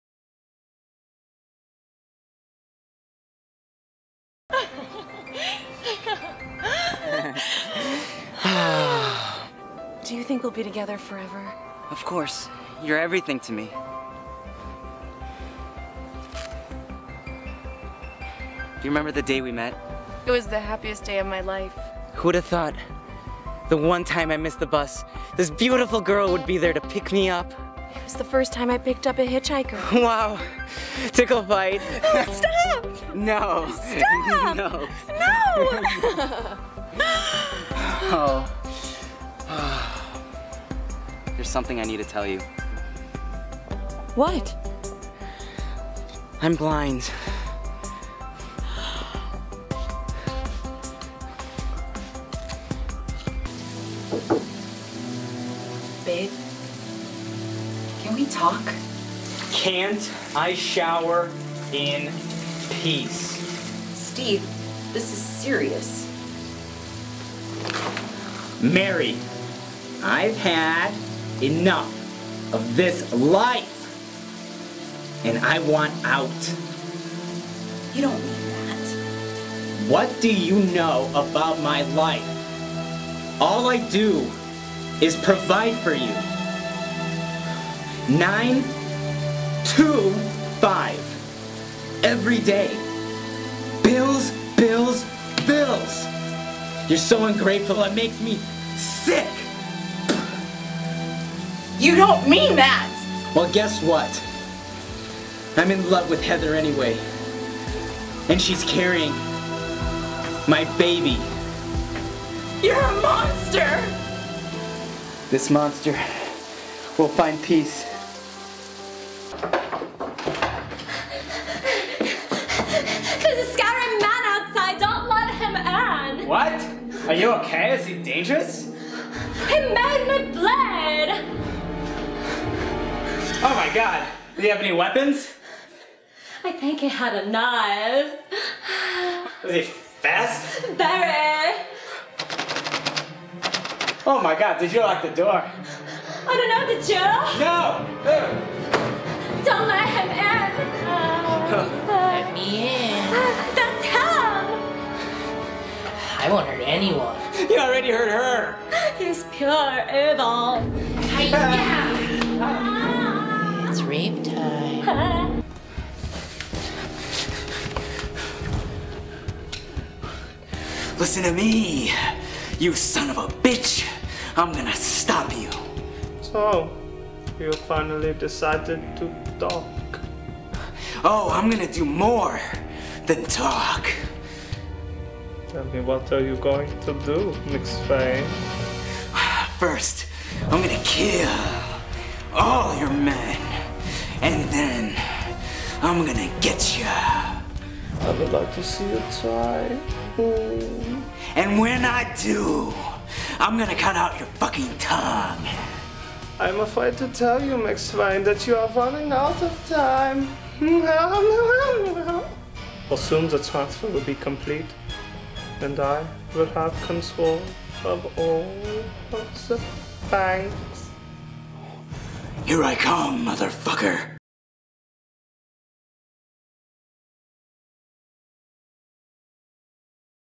Sam Milman Acting Reel | Bad Weather Films
Sam Milman's acting reel.

Bad Weather Films

Bad Weather Films is a sketch comedy duo consisting of high school friends Sam Milman and Peter Vass.